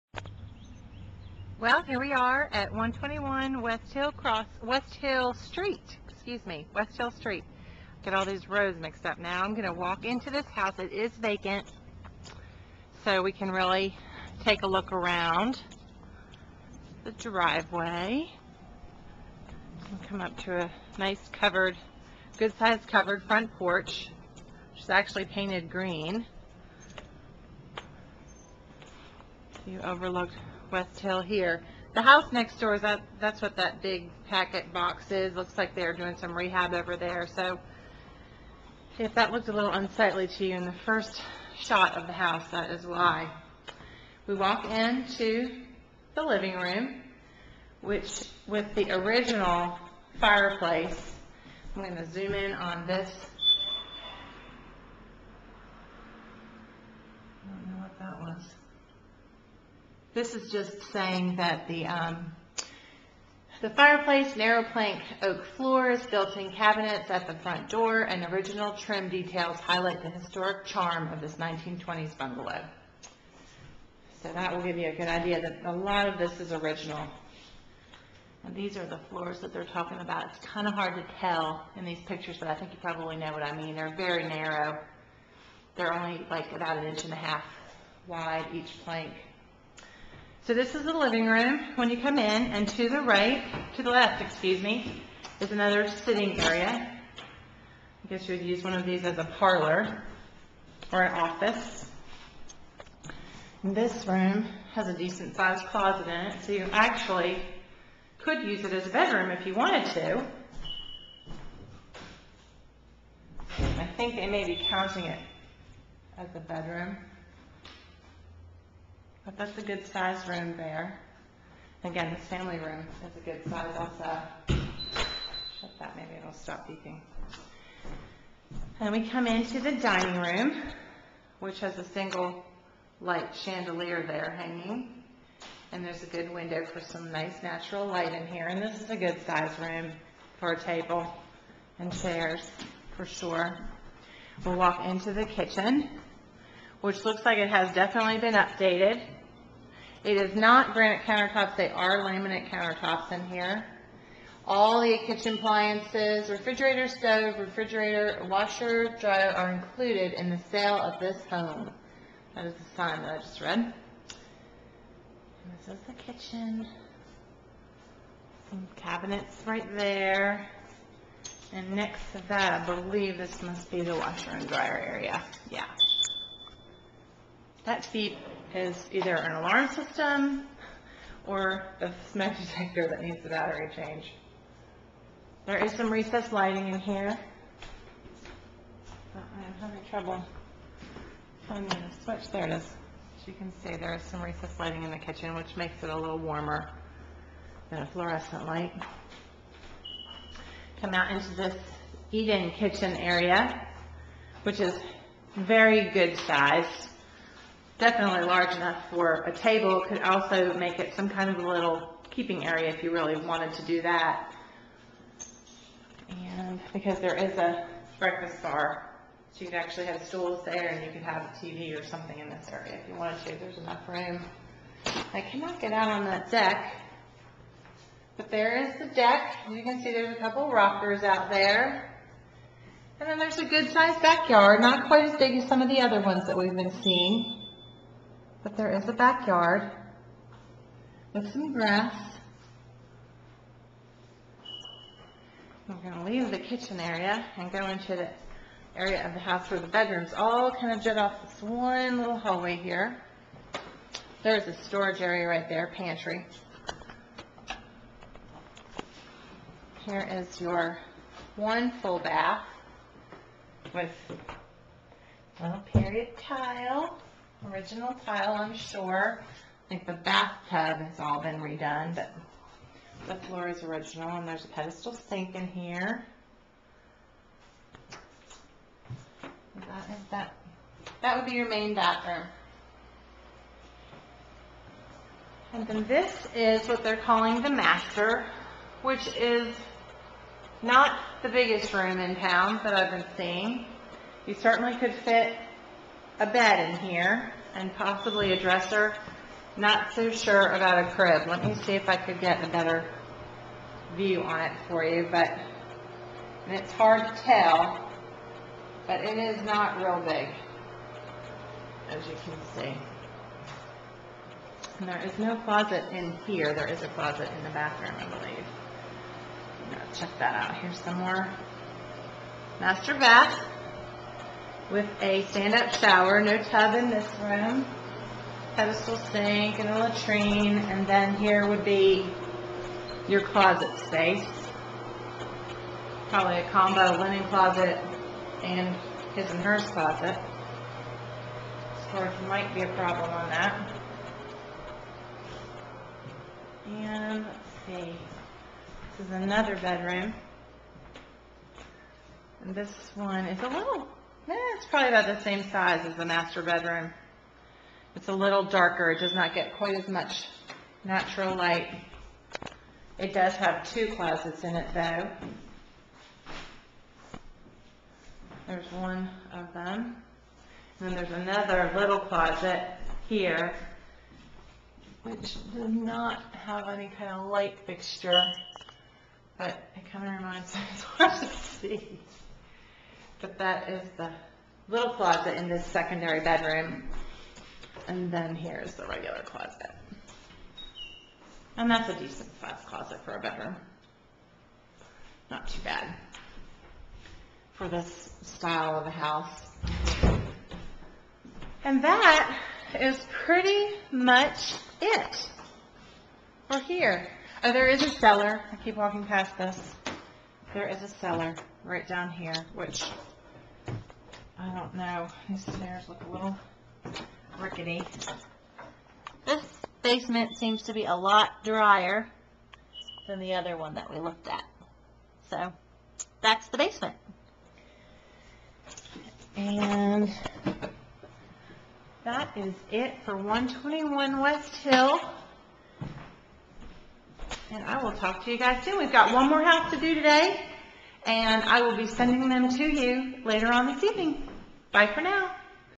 Duffy Realty Buyer's Agent Reeny Reynolds
Duffy Realty Buyer's Agent Reeny Reynolds previews a charming home for one of her clients.